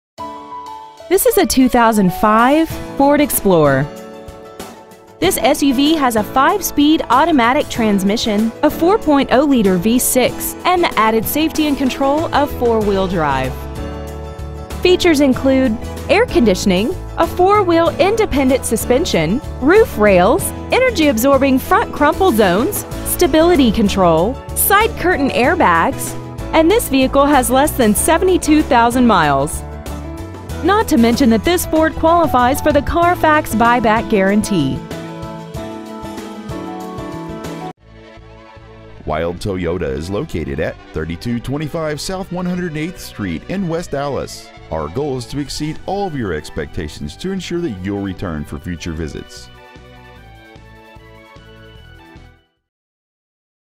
Pre-Owned 2005 Ford Explorer Milwaukee WI
Year : 2005
 Make : FORD TRUCK
 Model : EXPLORER
 Engine : Gas V6 4.0L/245
 Trans . : A
 Exterior : BLUE
 Miles : 71,428

 Stock : G60116

 3225 South 108th St.